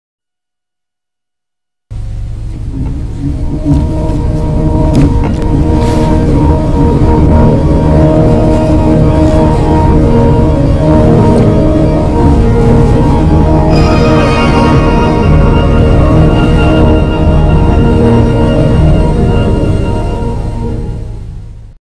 New World International Enhanced with Diamond Audio Effect (Requested by RieB79)
The Diamond effect spreads to this New World Logo.  I don't own the logo and claim no ownership.  Whoever owns New World's library now - it's theirs.  Requested by RieB79.